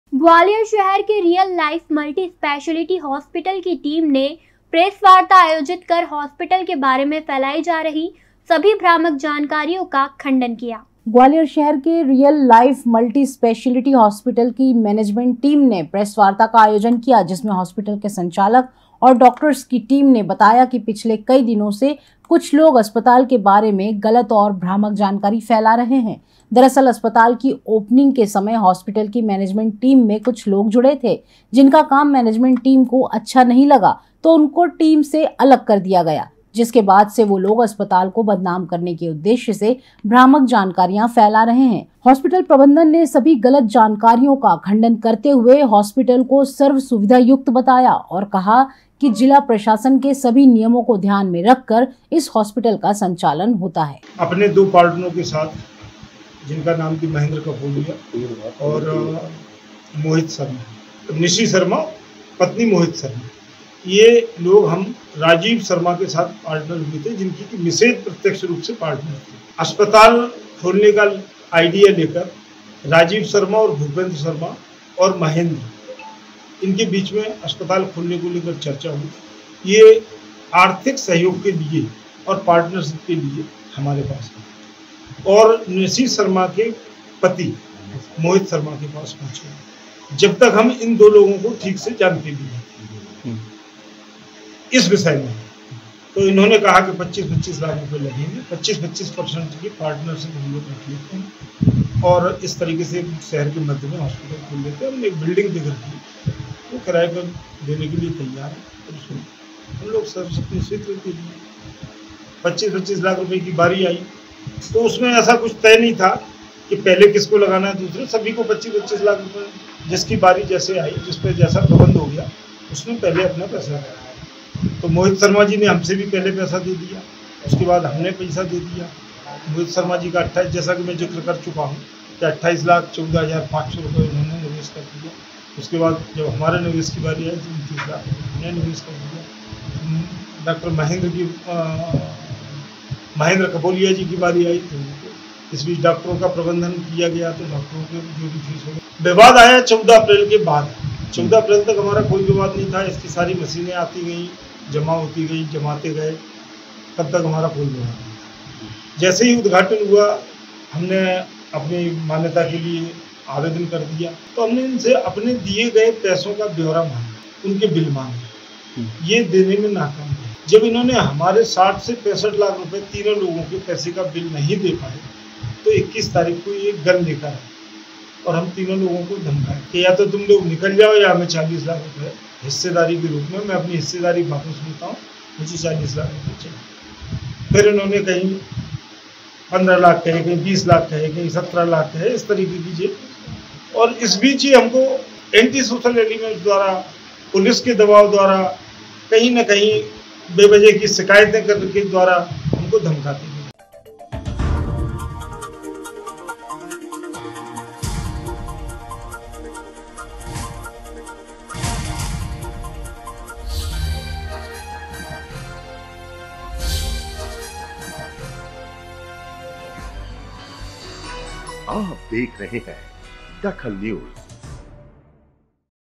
DAKHAL NEWS रियल लाइफ मल्टी स्पेशलिटी हॉस्पिटल ने की प्रेस वार्ता
रियल लाइफ मल्टी स्पेशलिटी हॉस्पिटल ने की प्रेस वार्ता
हॉस्पिटल को बदनाम करने वाले लोगो का किया खुलासा